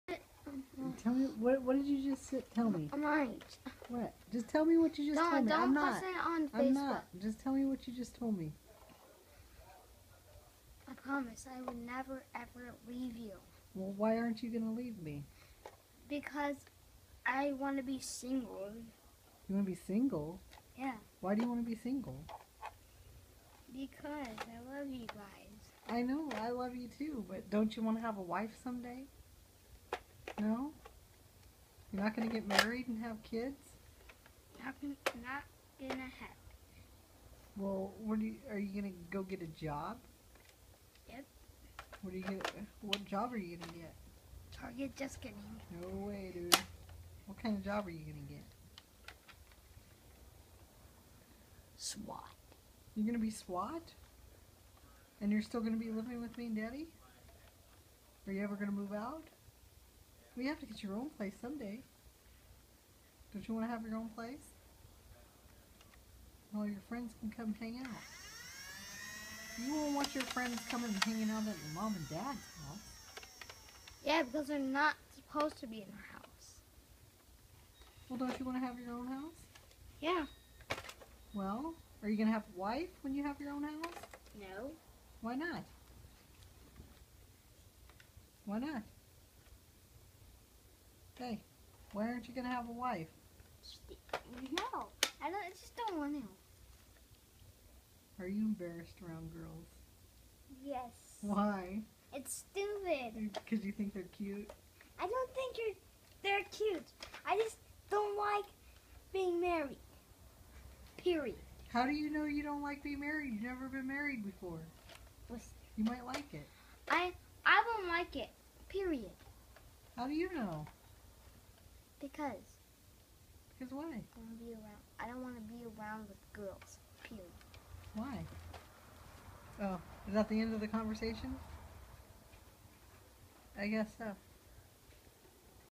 Mikey doesn't want to get married
Heart to heart conversation with my 7 year old son about jobs, moving away from home and getting married